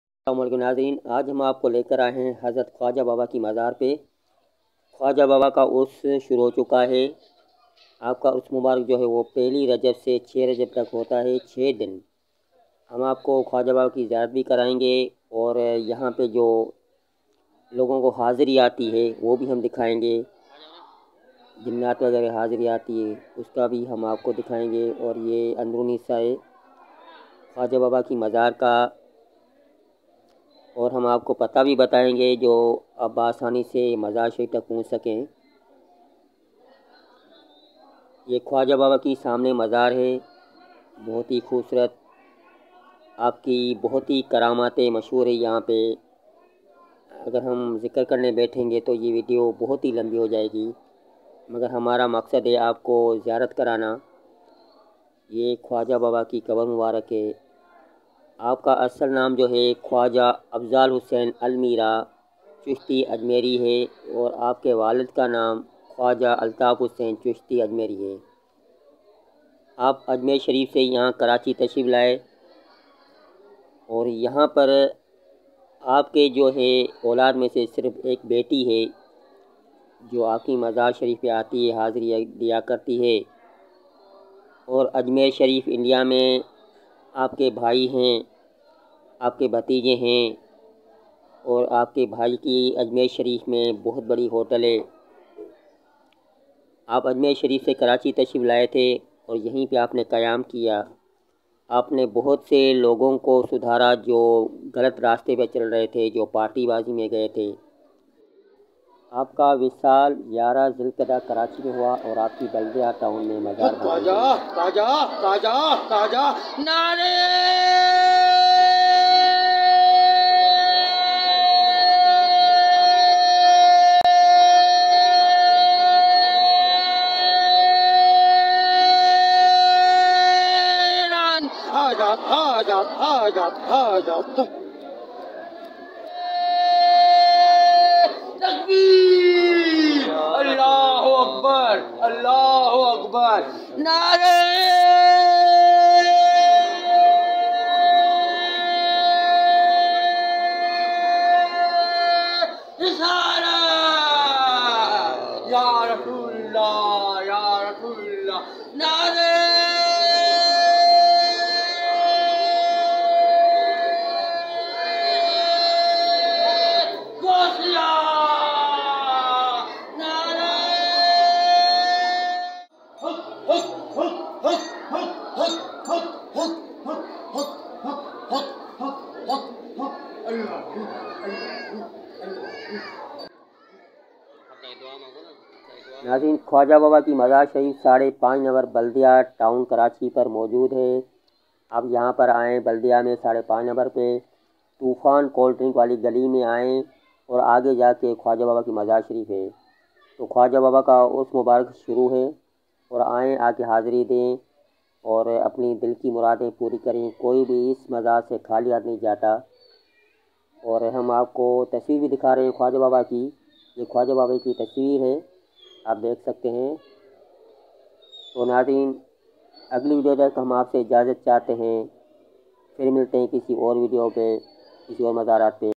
urs khuwaja baba 2022 dargah baldia town karachi || mazar khuwaja afzal husain ajmiri karachi | hira
is video me khuwaja baba ki ziyarat ki video he

urs
1 rajab se 6 rajab

adress
5/50 number baldya town karachi sindh

dargah alam shah

nana baba

nago shah